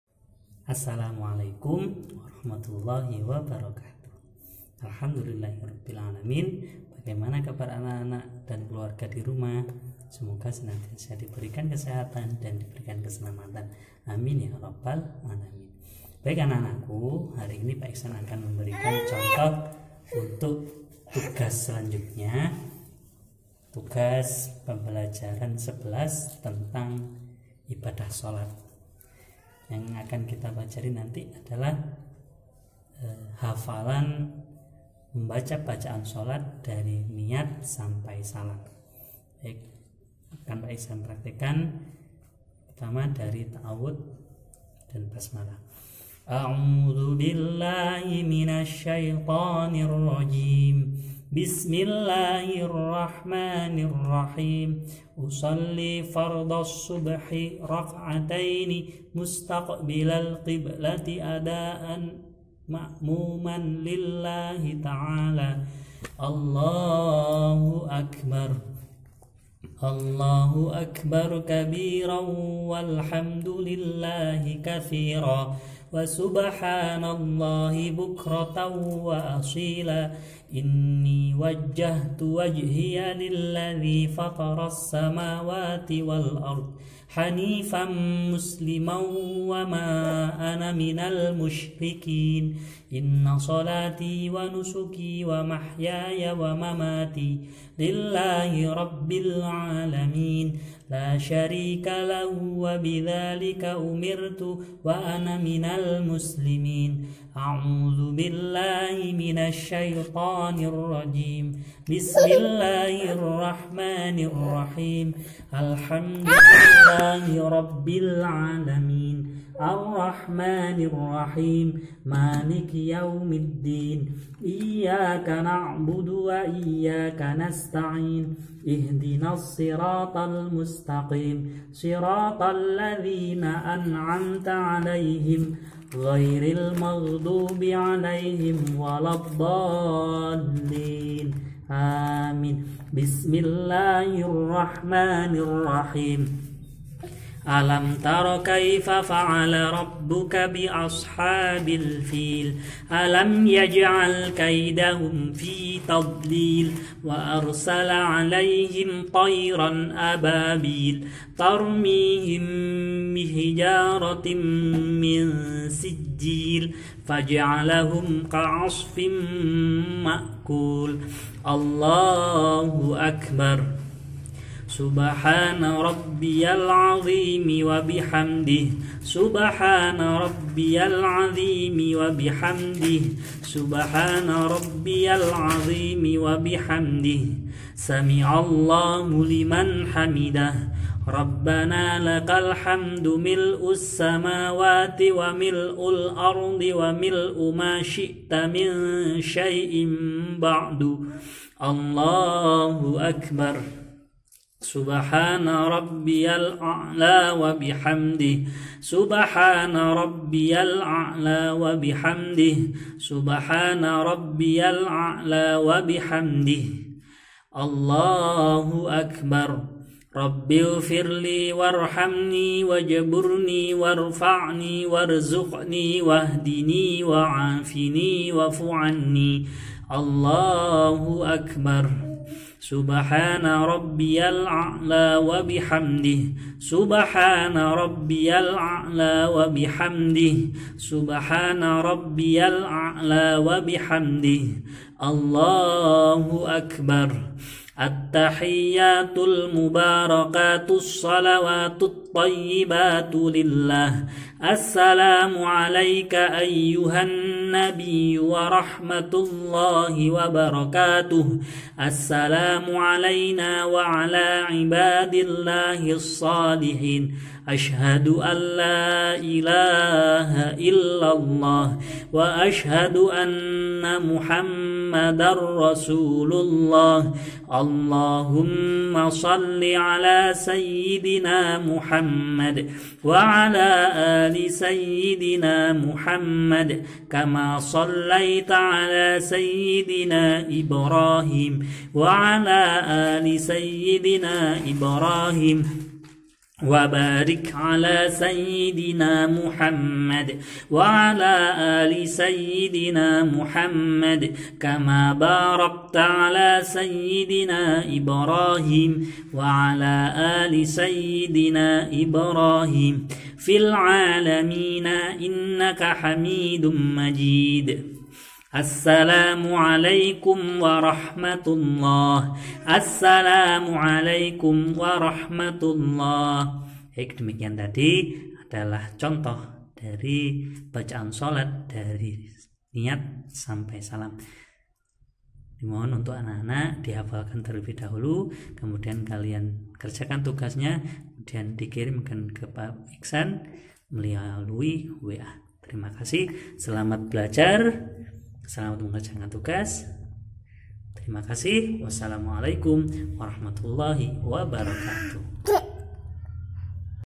PEMBELAJARAN PAI KELAS 3 DAN 4 CONTOH BACAAN SHOLAT 5 WAKTU
Mari kita belajar Materi PAI dan Budi Pekerti
Kurikulum 2013 dengan menyenangkan melalui video pembelajaran animasi POWTOON.
KELAS           : 4
TEMA            : 9
SUBTEMA     : 3

Created by : IKHSAN Media Interaktif

Mari bantu channel GURU BELAJAR PAI agar semakin berkembang,